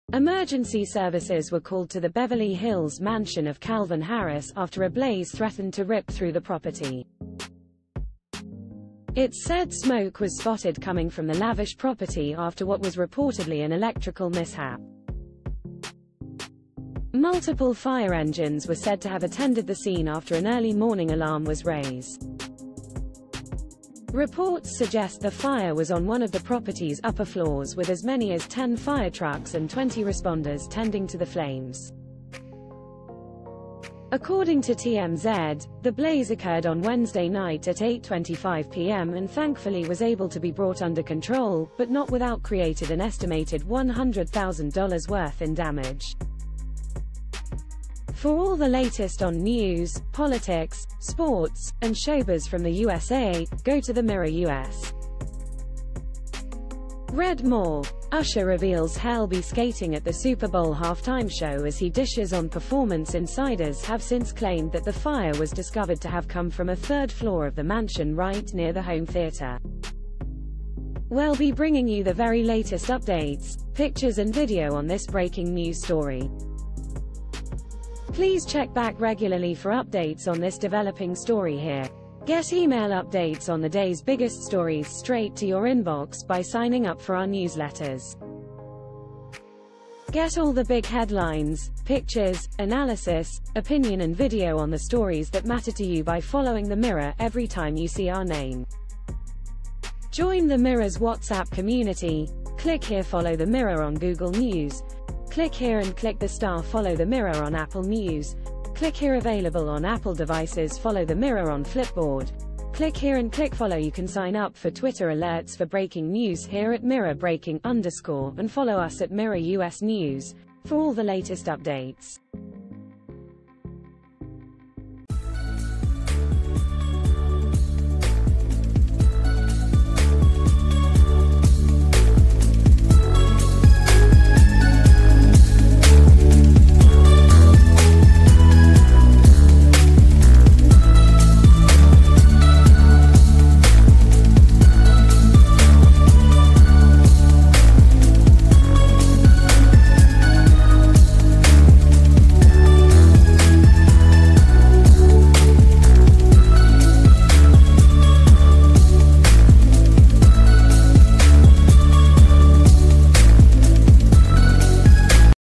Calvin Harris' mansion catches fire as 'electrical mishap' causes damage
Emergency services were called to the Beverly Hills mansion of Calvin Harris after a blaze threatened to rip through the property.  It's said smoke was spotted coming from the lavish property after what was reportedly an "electrical mishap". Multiple fire engines were said to have attended the scene after an early morning alarm was raise. Reports suggest the fire was on one of the property's upper floors with as many as 10 fire trucks and 20 responders tending to the flames.  According to TMZ, ...